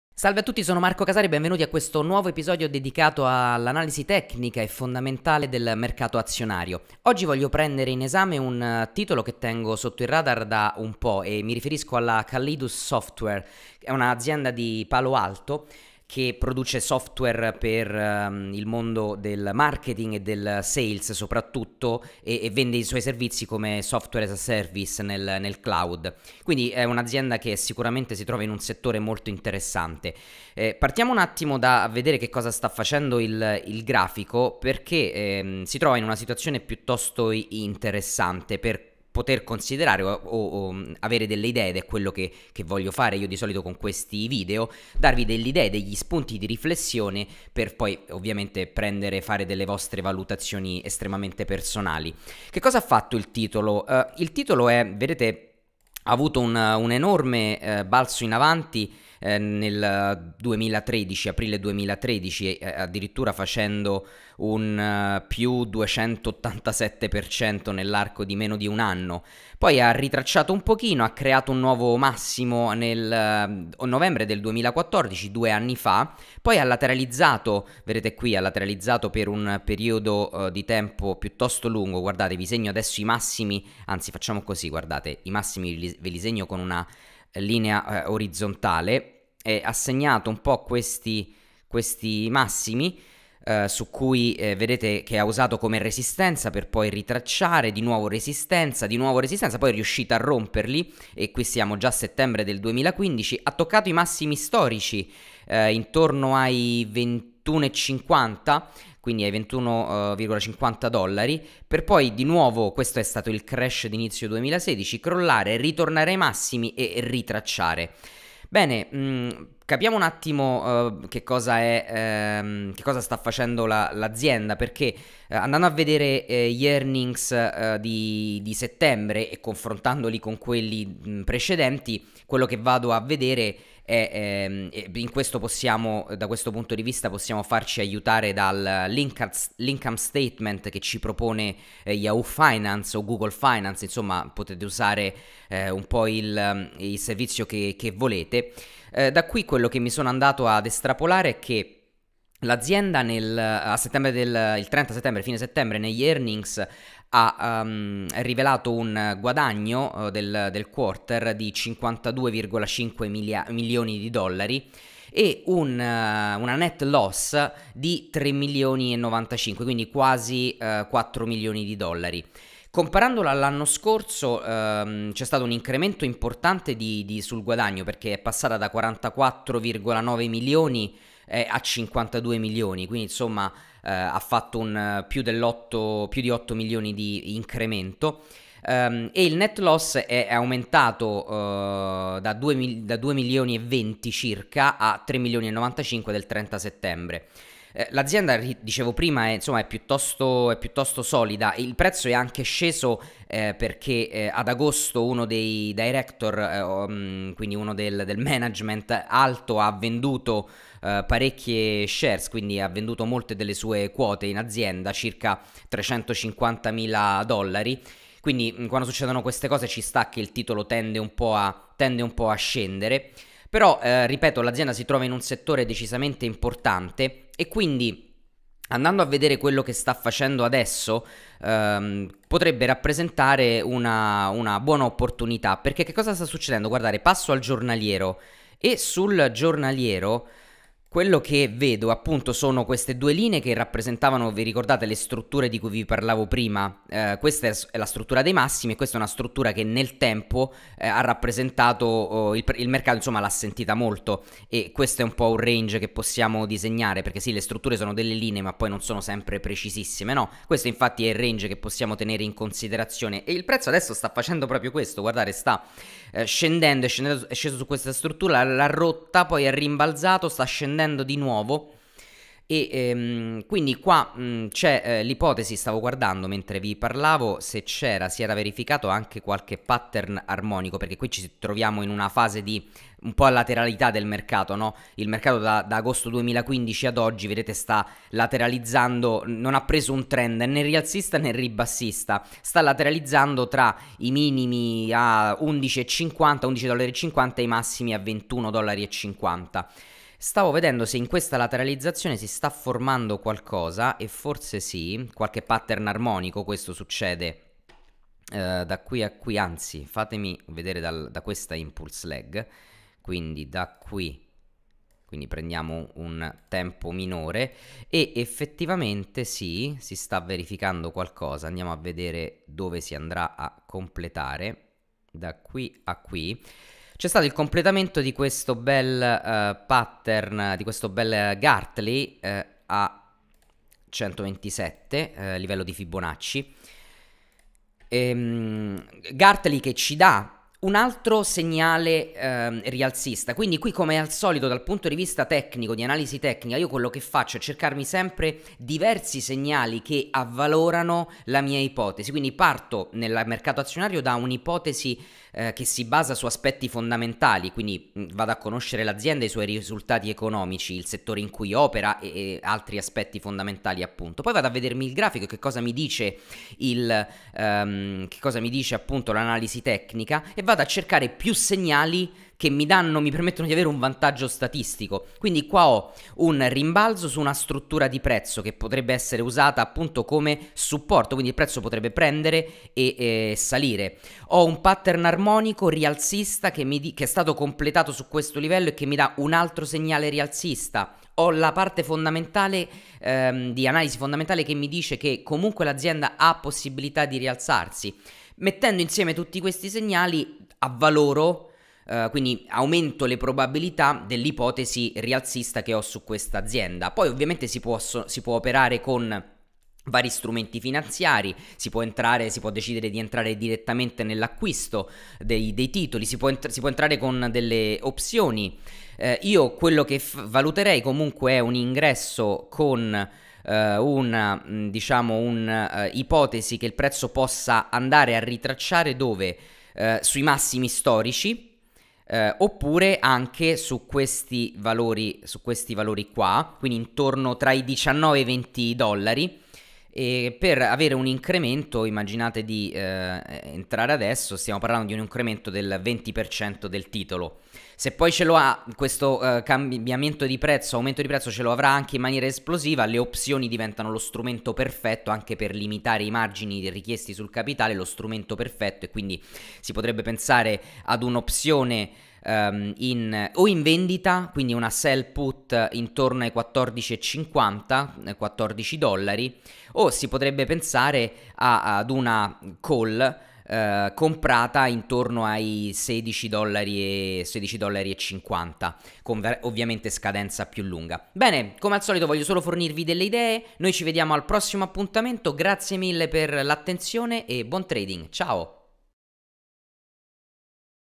Analisi fondamentale e tecnica sull'azionario: come trovare le opportunità migliori
Ricevi gratuitamente gli studi di analisi tecnica ogni settimana sui mercati finanziari.

Analisi Tecnica e Fondamentale applicata al mercato azionario per una strategia vincente sulle azioni.

In questo video faccio analisi sul titolo Callidus Software (ticker:CALD).
Ecco l'agenda completa del video:
00:56 -- Trend rialzista del 2013 sul titolo azionario
01:30 -- Formazione di una struttura di prezzo usata come resistenza
01:45 -- Massimi storici toccati sui $21.5
02:05 -- Analisi fondamentale sugli earnings di Settembre 2016 mettendolo a confronto con quelli del 2015
02:40 -- GAAP net loss e fatturato
04:00 -- Analisi tecnica su grafico giornaliero
04:20 -- Range dei minimi usato come supporto
04:40 -- Utilizzo del ritracciamento di Fibonacci per cercare un possibile  pattern armonico
05:05 -- Completamento di un pattern armonico Gartley